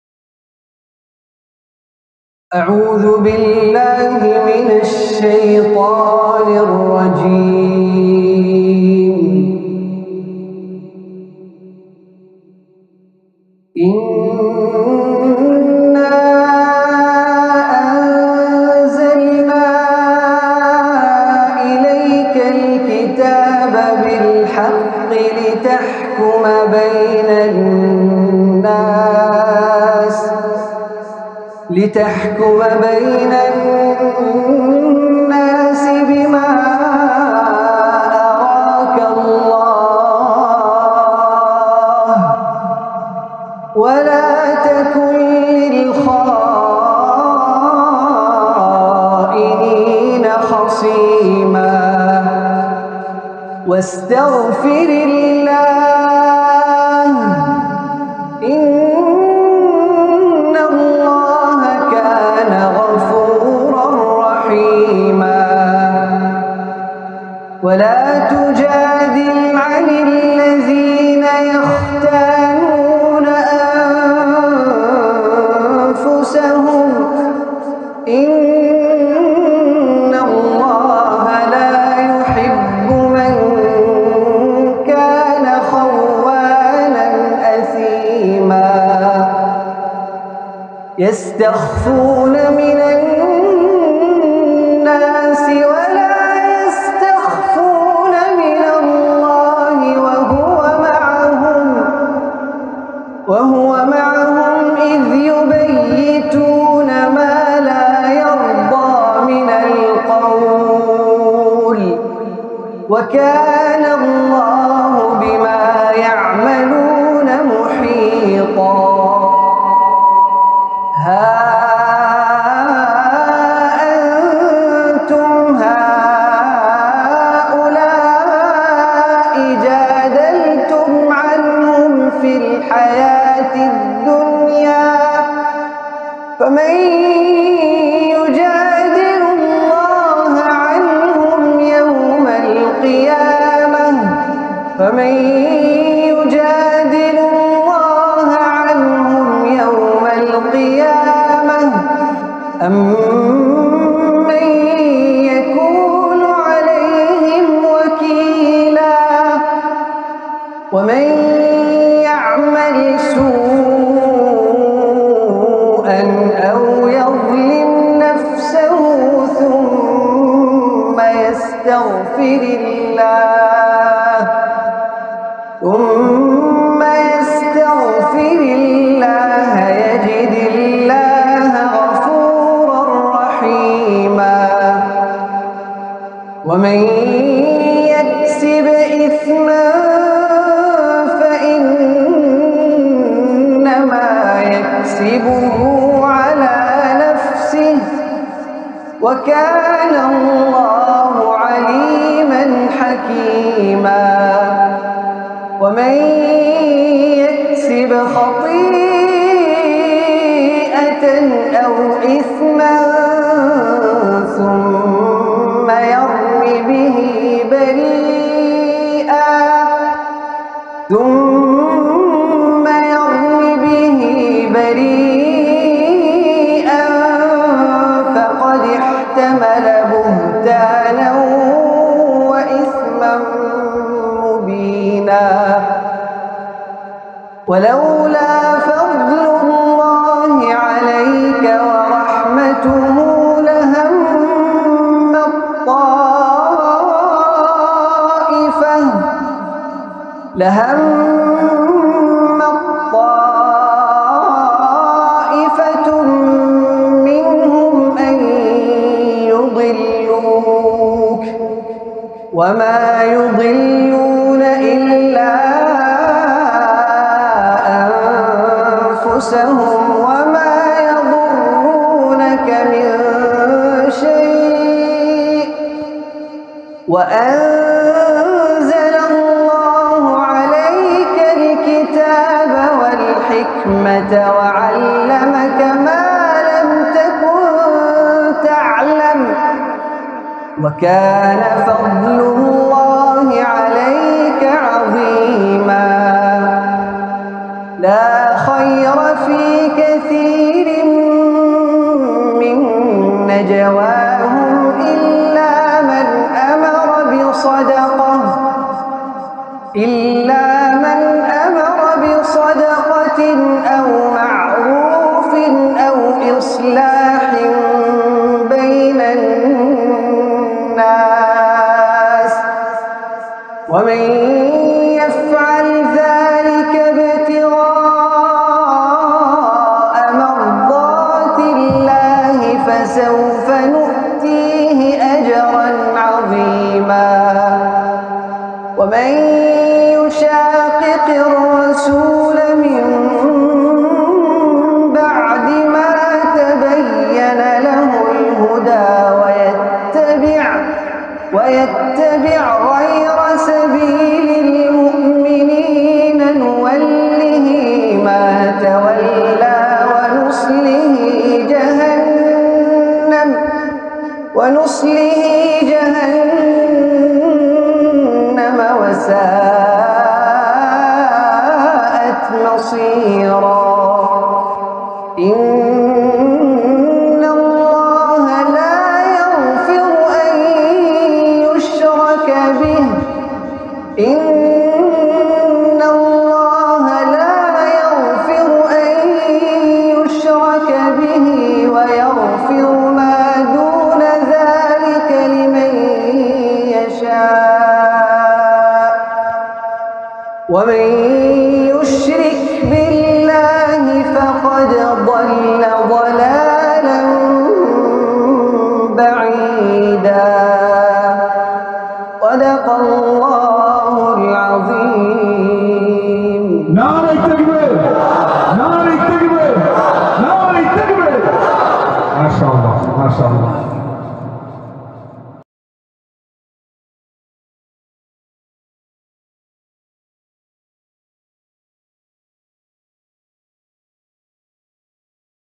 International Qirat conference 2022. Basirhat West Bengal India.
International Qirat conference 2022.Basirhat West Bengal India. Qari Syed Miqdad Saudi Arab.vocal editing M.H Islamic tube home studio. video editing and mixing Minhaz Halder.